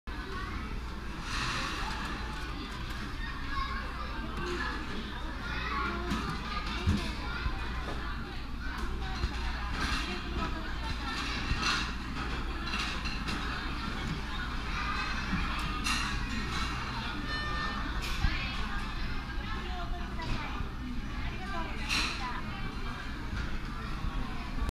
Robot Bartender - Automatic beer pouring machine
Robot Bartender
Automatic beer pouring machine
Source video, image and text: Dr. Brandon Wiley